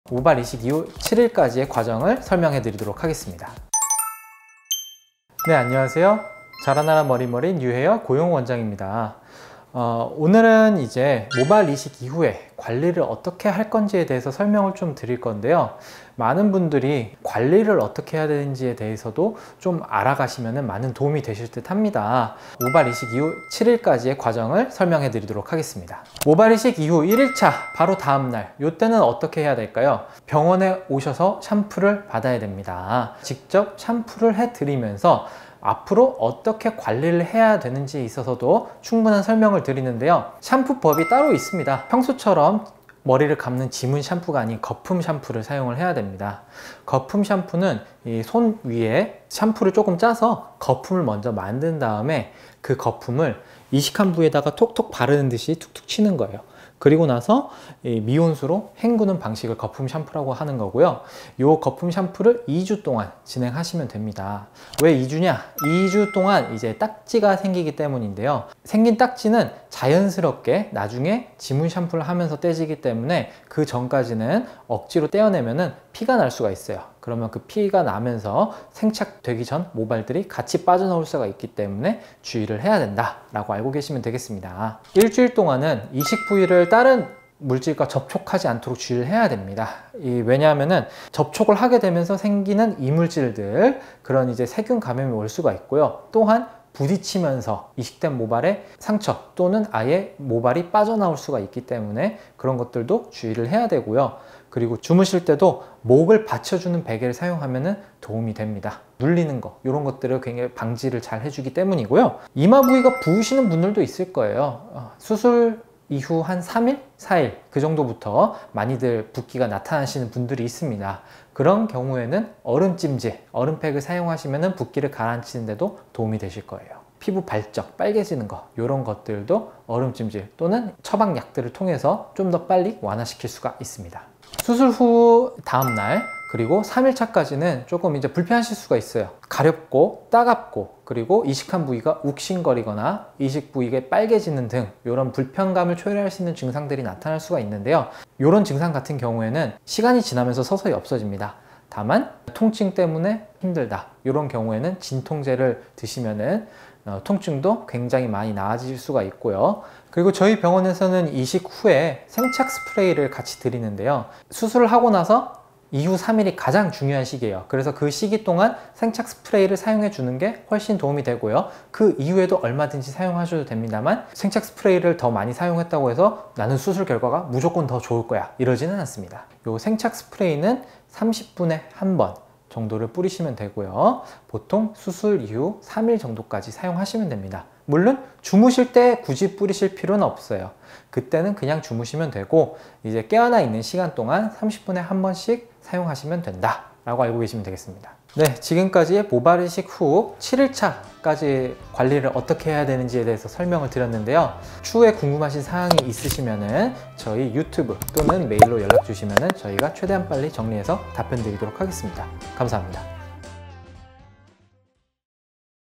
모발이식하고 7일 동안 이렇게 관리하면 좋습니다ㅣ뉴헤어
모발이식 후 3일은 생착을 결정하는 굉장히 중요한 시기이기 때문에
관리를 잘 하는 것이 중요합니다.
그래서 오늘은 모발이식 후 7일 동안 어떻게 관리를 해야 하는지 알려드리겠습니다.

💖타임라인
0:00 시작
0:26 모발이식 다음날, 샴푸 방법
1:05 모발이식 후 거품 샴푸를 하는 이유
1:25 모발이식 후 7일간 주의사항
2:15 이식부위의 통증 완화 방법
2:40 생착 스프레이 사용법